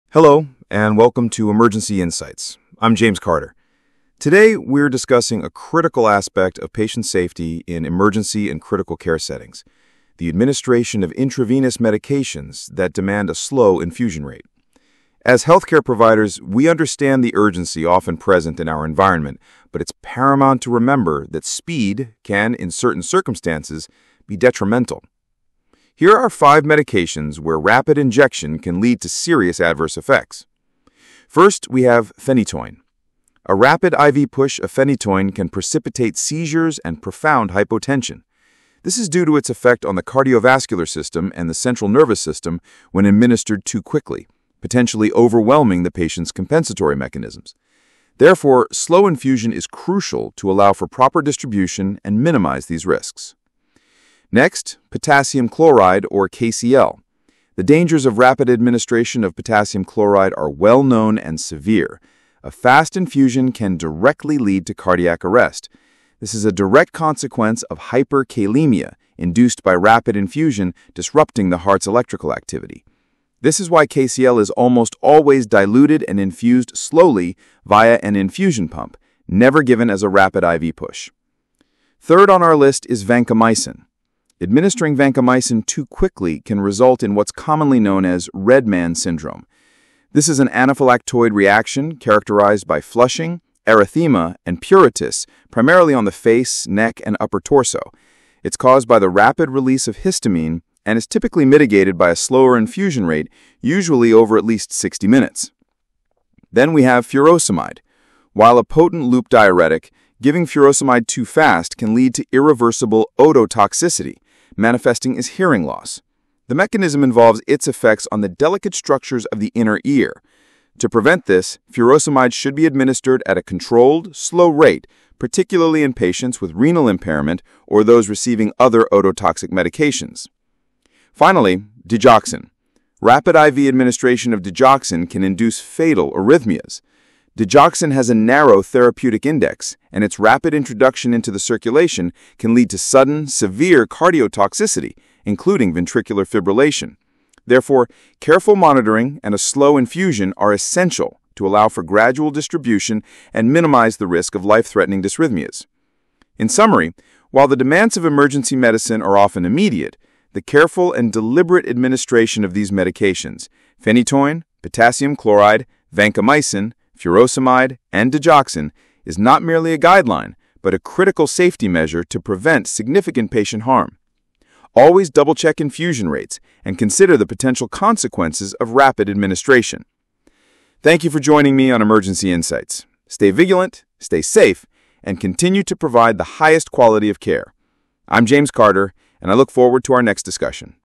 Slow Injection Medications
IV DRUGS THAT MUST BE GIVEN SLOWLY
- Rapid injection: serious adverse effects

Phenytoin
- Antiepileptics
- Rapid push: seizures, hypotension

Potassium Chloride (KCl)
- Treat hypokalemia
- Rapid IV: cardiac arrest

Vancomycin
- Antibiotics covering MRSA
- Fast: Red Man Syndrome

Furosemide
- Diuresis
- Too fast: hearing loss

Digoxin
- Antiarrhythmics
- Rapid IV: fatal arrhythmia